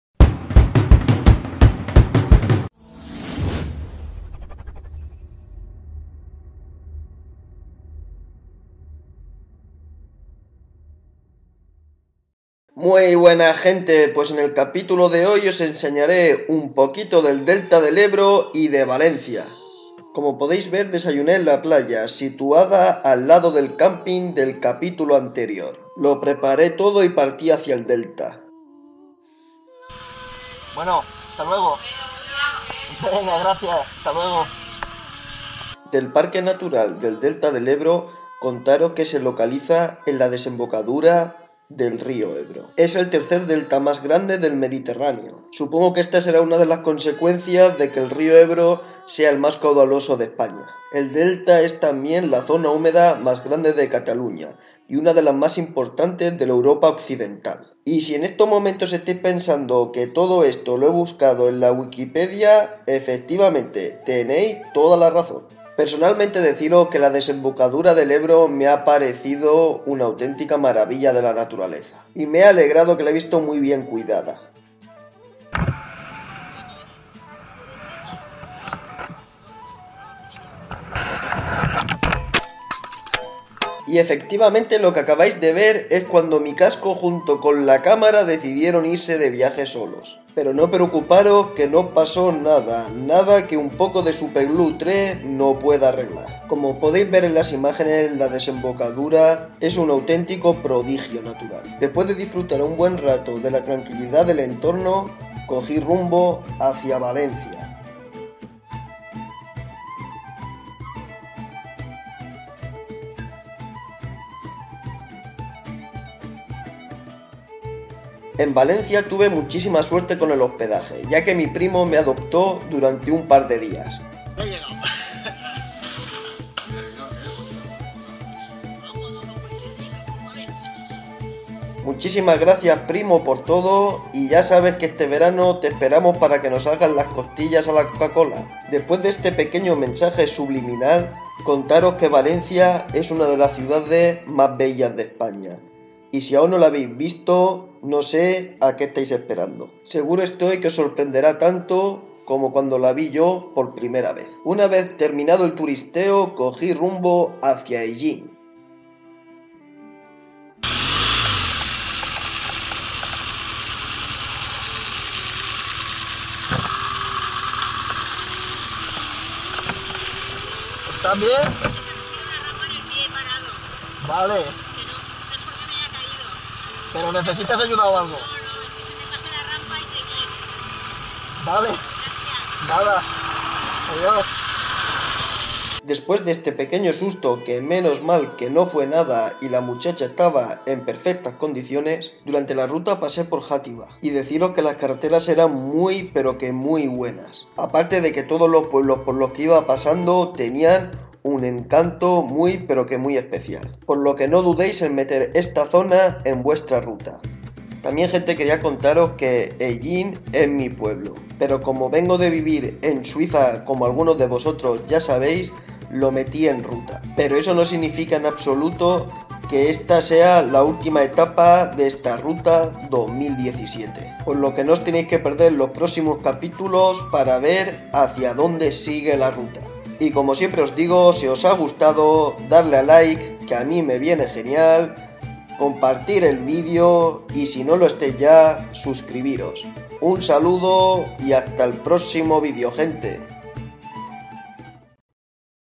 CAMARA Y CICLISTA AL SUELO!!! Capitulo 7 Delta del Ebro y Valencia
Ruta en moto por el Delta del Ebro y Valencia